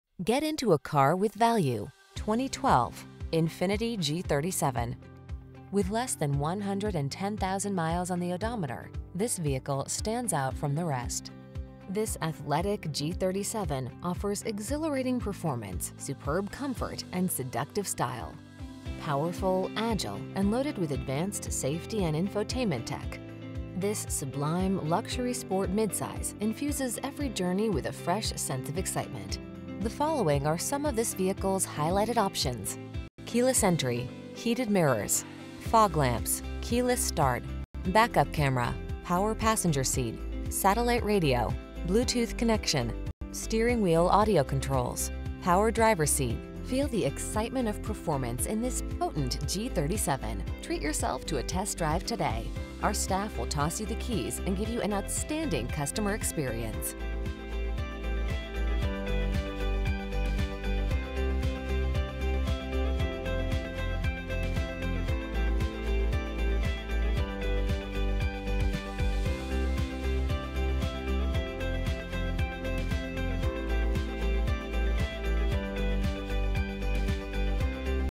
2012 INFINITI G37_Convertible Base Orlando, Winter Park, Clermont, Merritt Island, Tampa FL
Pre-Owned 2012 INFINITI G37_Convertible Base available at Orlando Infiniti located in 4237 Millenia Blvd, Orlando, FL, 32839.
Stock#: U202968 - VIN#: JN1CV6FE9CM202968

Servicing the Orlando, Winter Park, Clermont, Merritt Island, Tampa area.

Get into a car with value, the   2012  INFINITI G37.  This athletic G37 offers exhilarating performance, superb comfort, and seductive style. Powerful, agile, and loaded with advanced safety and infotainment tech, this sublime, luxury sport mid-size infuses every journey with a fresh sense of excitement. The following are some of this vehicles highlighted options: Keyless Entry, Heated Mirrors, Fog Lamps, Keyless Start, Back-Up Camera, Power Passenger Seat, Satellite Radio, Bluetooth® Connection, Steering Wheel Audio Controls, Power Driver Seat. Feel the excitement of performance in this potent G37. Treat yourself to a test drive today. Our staff will toss you the keys and give you an outstanding customer experience.